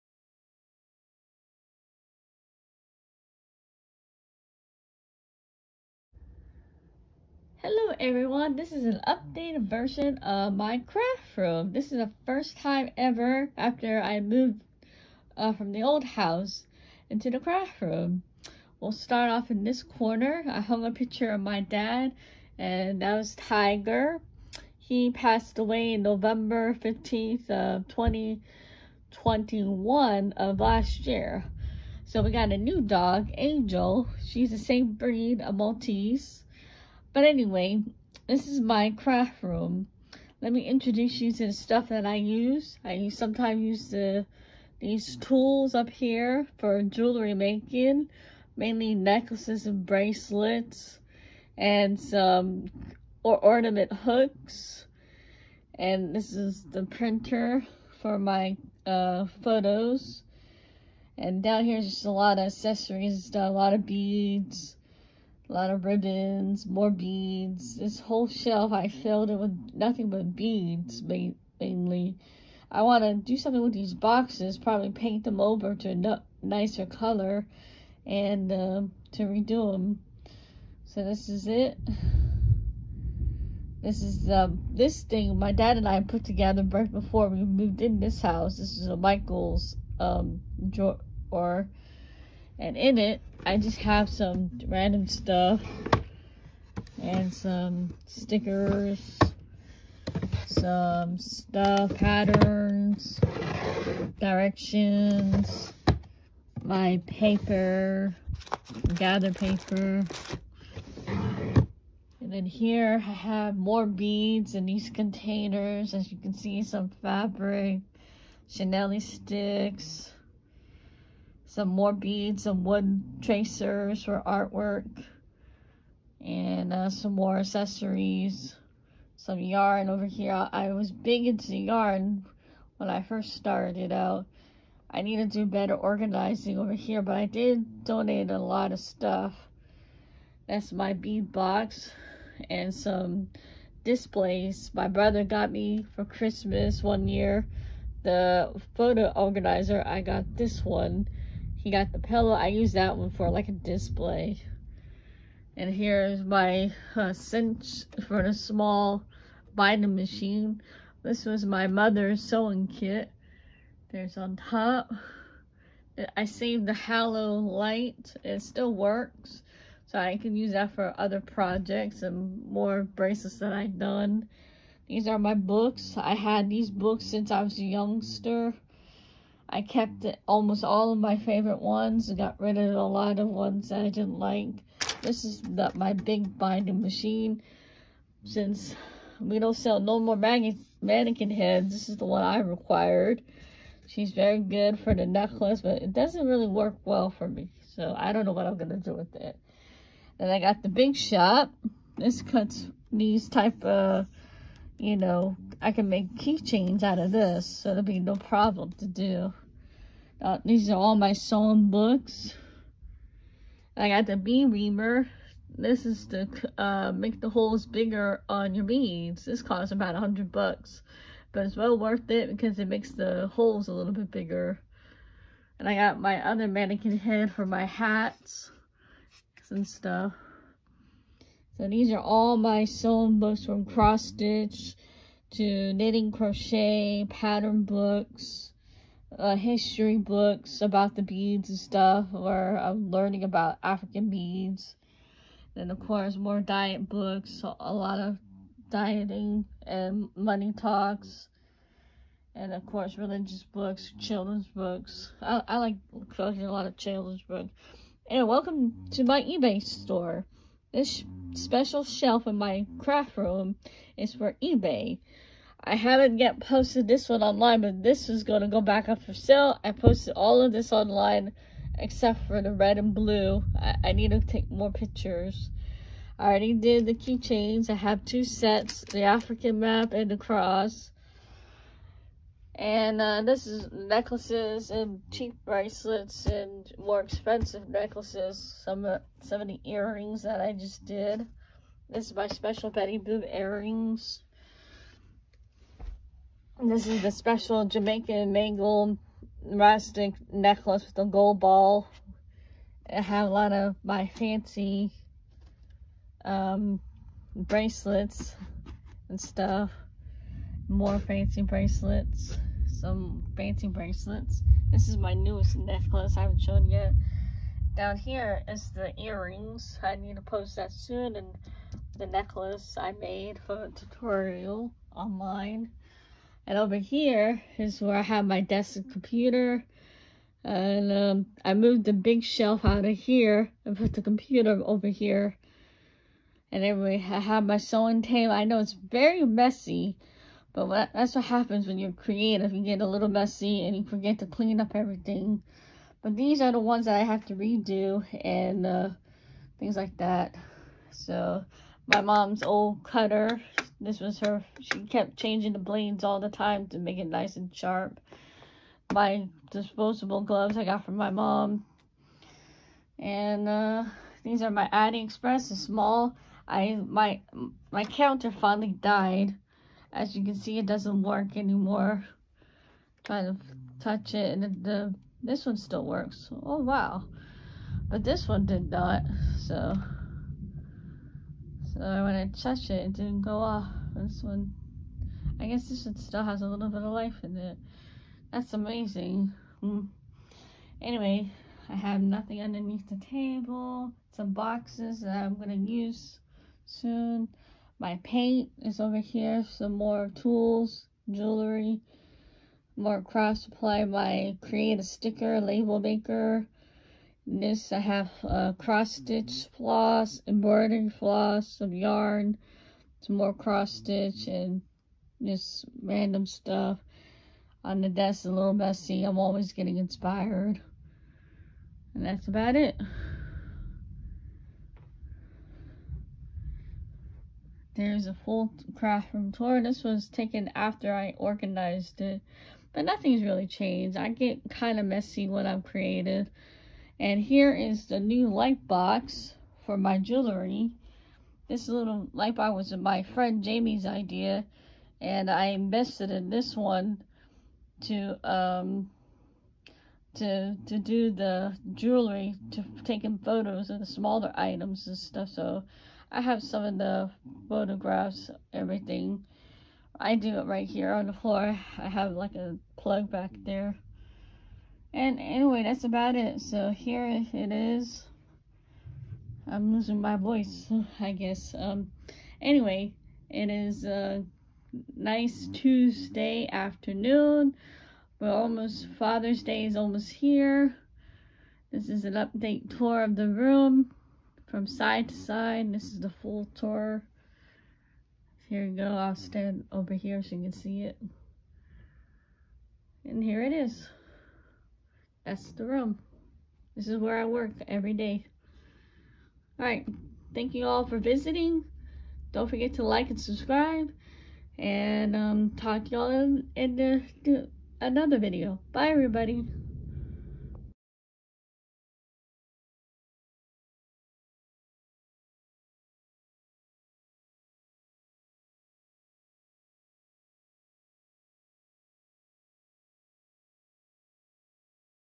SewingforHobby Craft Room Tour
This is a new updated tour of the craft room.  In this adventure, SewingforHobby shows her craft room.  This where she creates her jewelry and doing stuff...

Kid friendly video